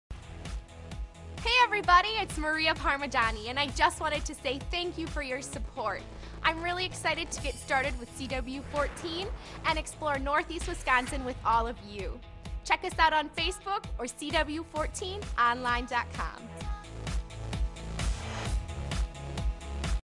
Maria Introduces herself
Maria Parmigiani introduces herself as the first CW 14 Star